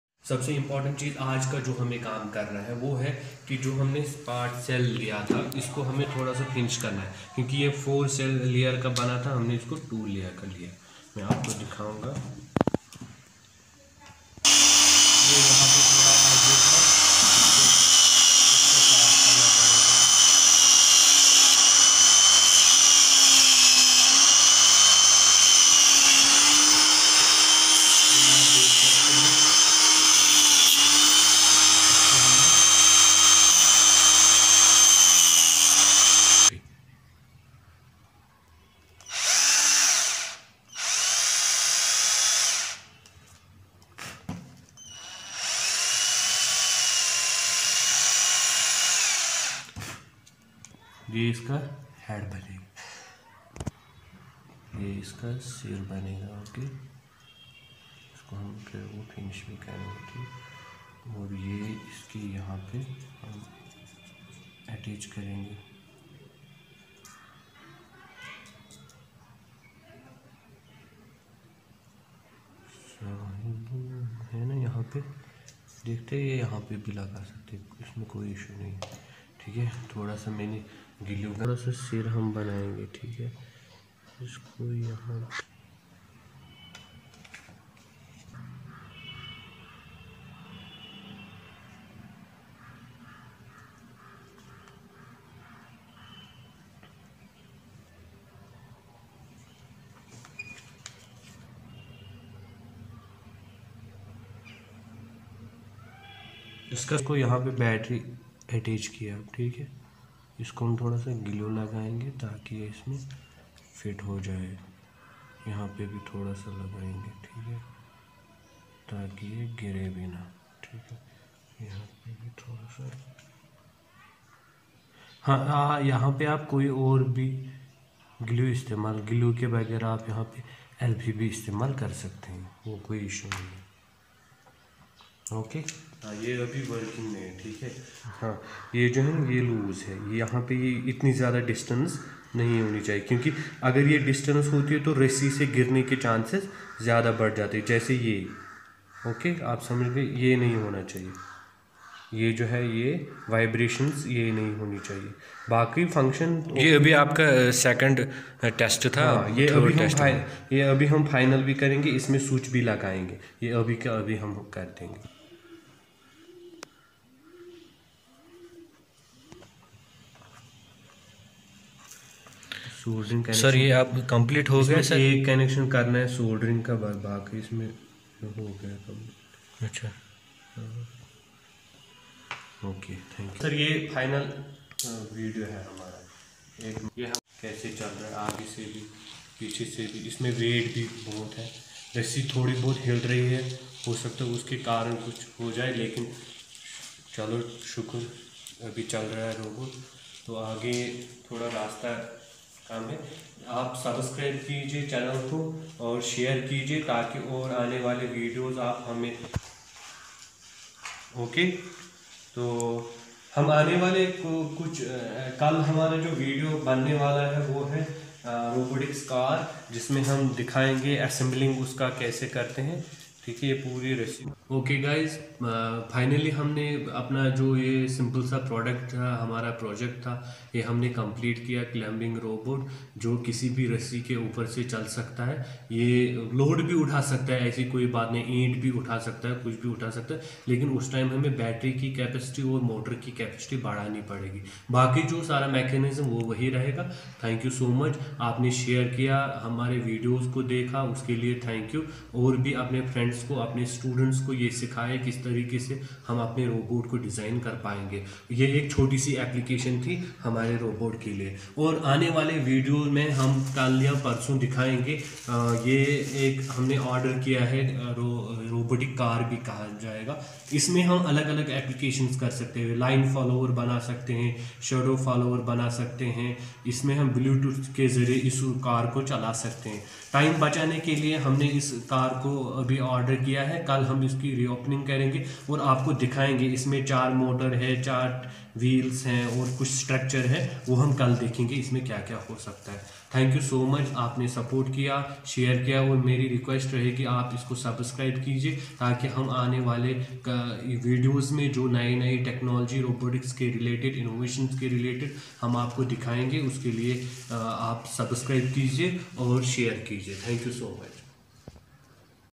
Finally Done Climbing Robot ❤❤@ Creativetwins jk.
Finally Done Rope Climbing Robot 🤖
Total 4 parts at Channel Creativetwins jk.
Inventor Mr. Refaz Ahmad Wani
Camera Man :- Mr Ishfaq Ahmad Wani
Thanks so much YouTube F